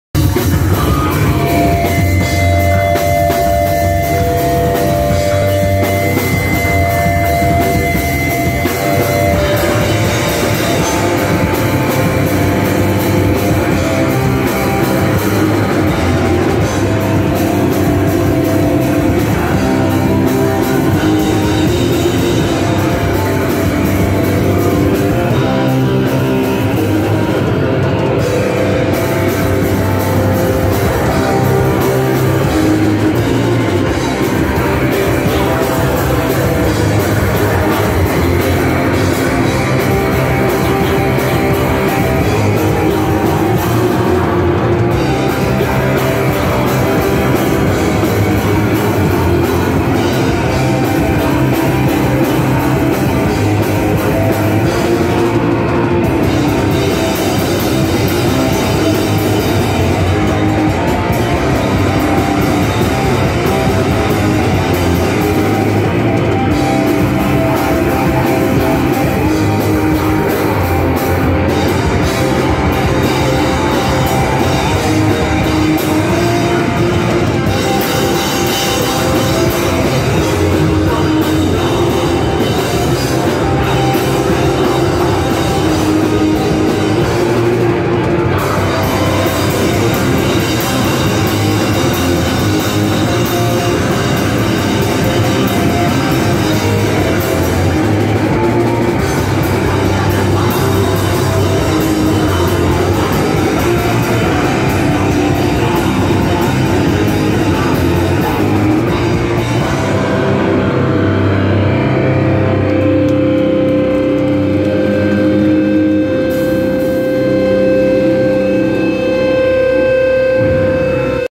20220625 - Interadd Part 1 from Gigs Akhir Bulan #10
Nama band: Interadd
Kota: Batu
Acara: Gigs Akhir Bulan #10
Tanggal: 25 Juni 2022
Tempat: Warkop Hippies, Yogyakarta